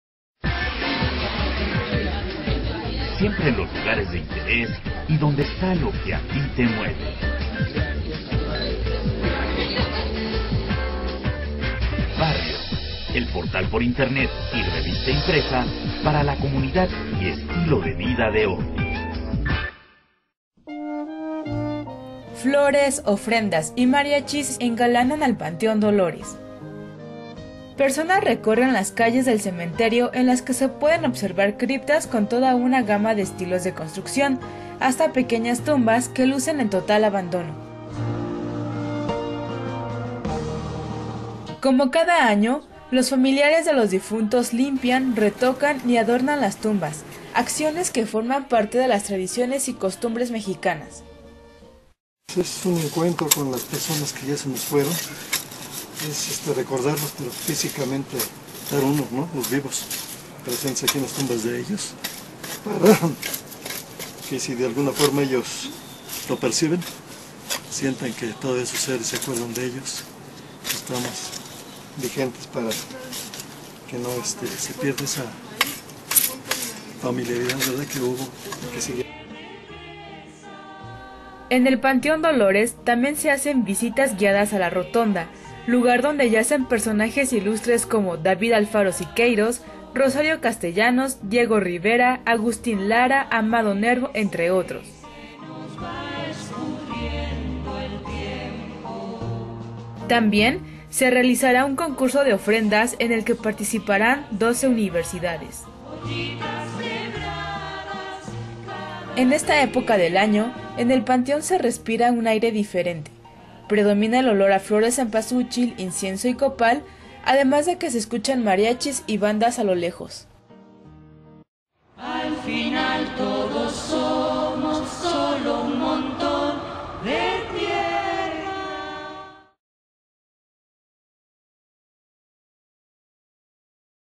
Panteón Dolores en el día de muertos
BARRIO realizó un recorrido por el Panteón Dolores